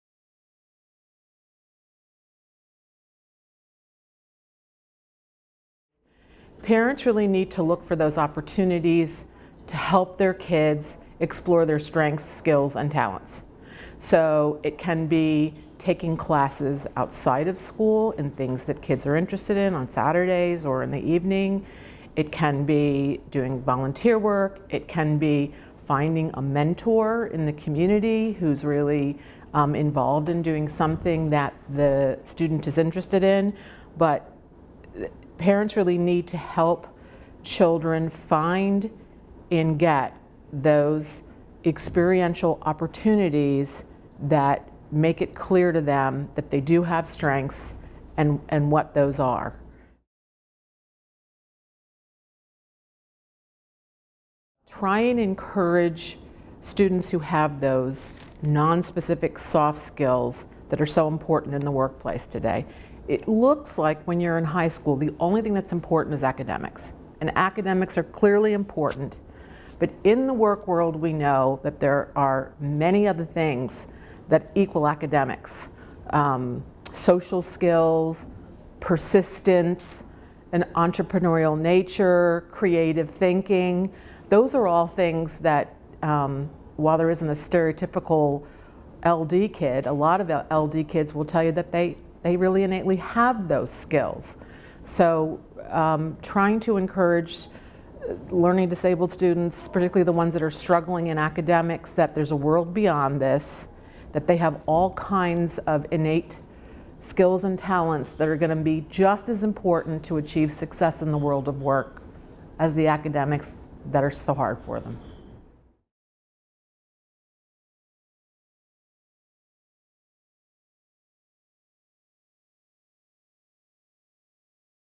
How Can I Prepare My Teen with LD for the Workplace?
Parents often struggle with helping their teens with LD choose a career path. Laura Breeden, an expert in career management, shares her expertise on how parents can help their children prepare for the workplace and what skills provide keys to success.

To read more about this important topic, visit our "Teen & Transition"